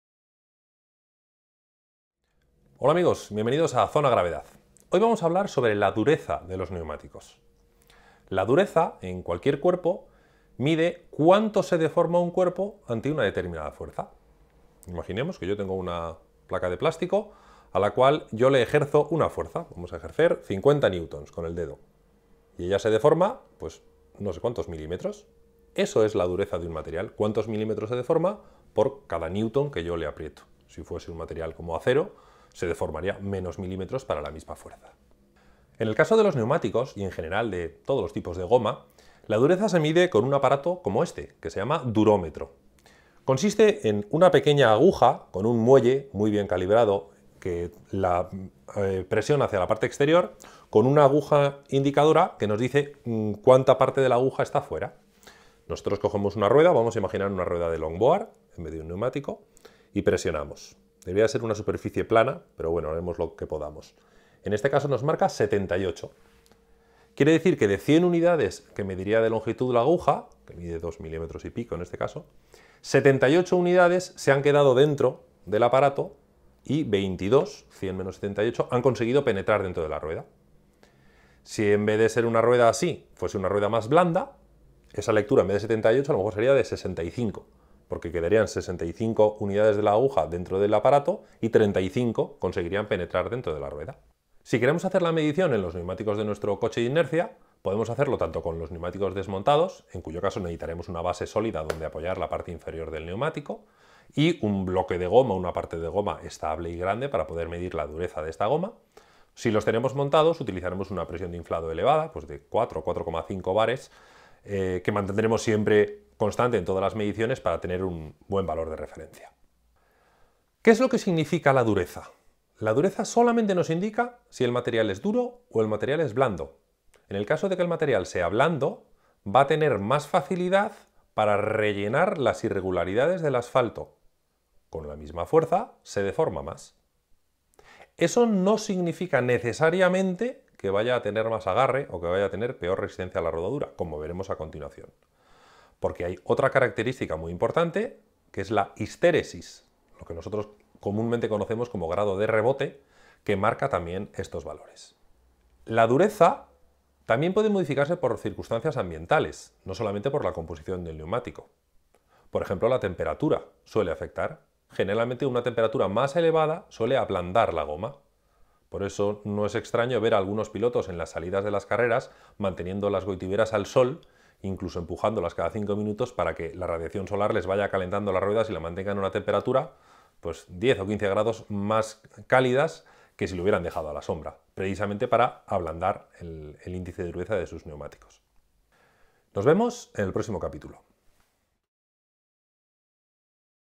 Dinámica 4: Dureza del neumático
Cómo se mide la dureza de un neumático y qué implicaciones tiene.